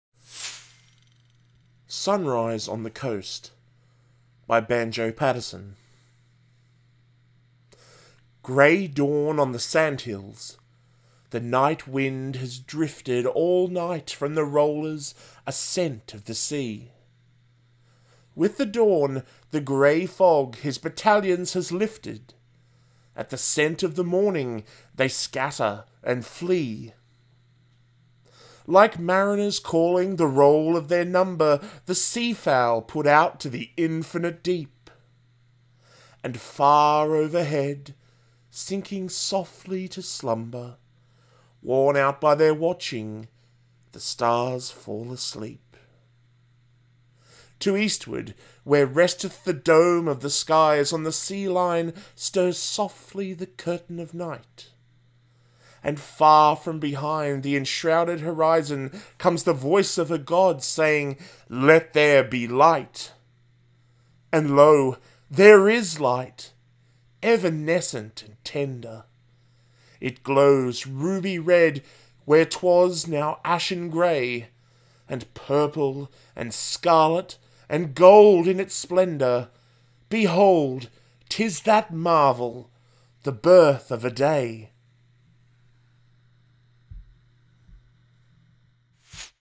Andrew recites: Sunrise On The Coast by Banjo Patterson
Author:  A.B. ‘Banjo’ Patterson
Type:  Lyric Poem, Australian Poem
Suitable:  12+

Andrew recites various prose, poetry, plays, and other significant writing from the wonderful world of literature.  Andrew resides in Queensland, Australia and is a Speech & Drama teacher.